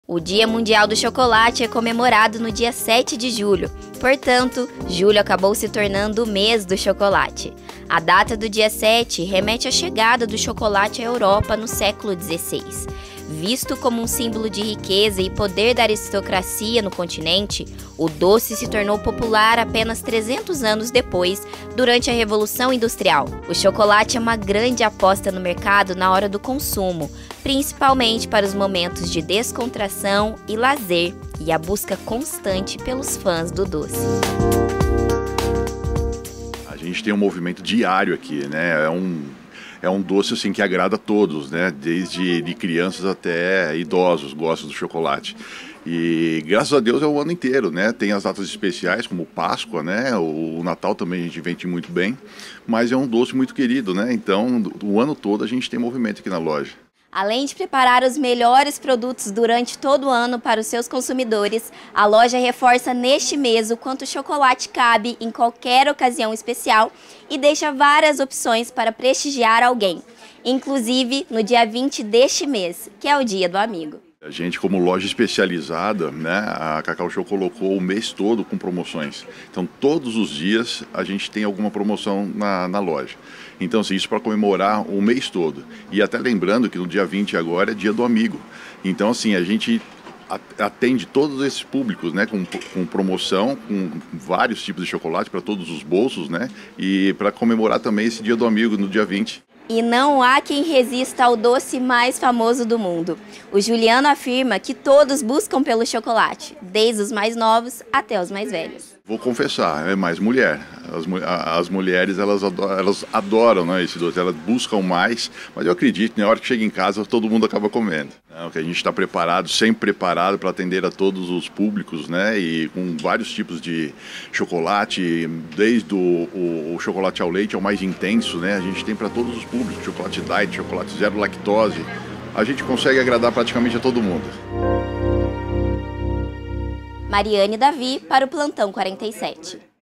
MÊS DO CHOCOLATE É COMEMORADO PELO COMÉRCIO DE POÇOS DE CALDAS
Em comemoração ao mês do chocolate, chocolaterias e lojas de chocolate fazem promoções e ofertas no comércio de Poços de Caldas.